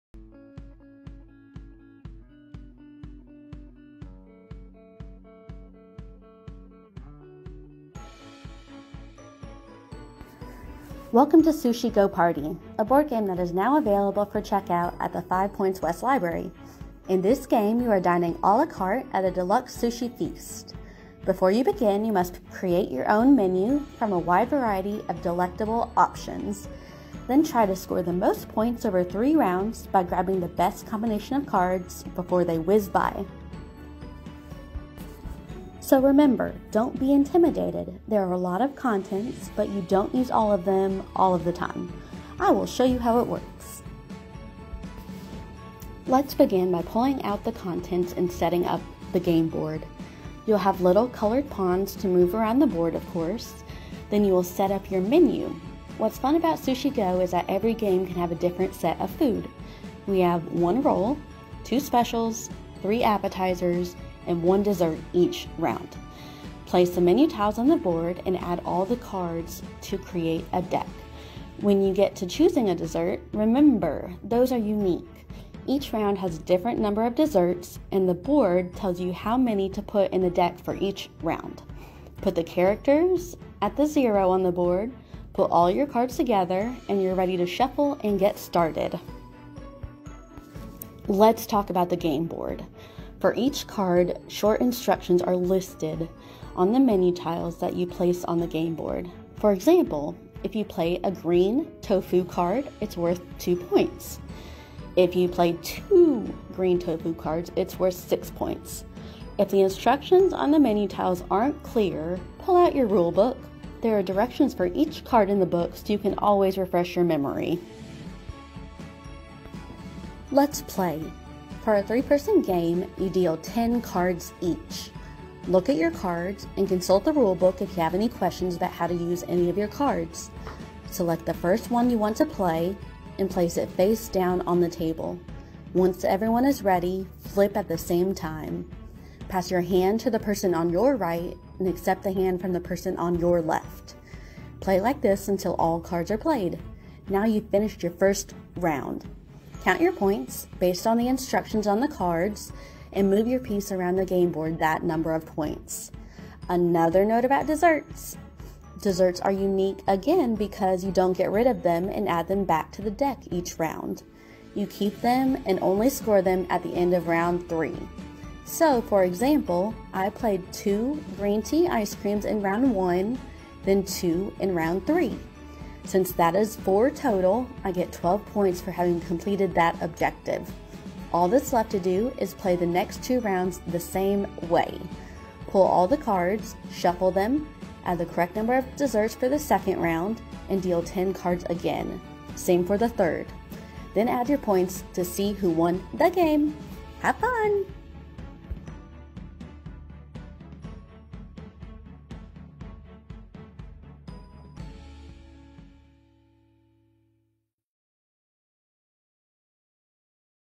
How to Play Sushi Go Party: Board Game
Get a Clue: Board Games w/ the Library
BPL has board games for all ages available, and we will even teach you how to play them. Avoid the family fight over the game rules by watching librarians play them first! Please visit the Birmingham Public Library's website to check out this and other board games for your next game night.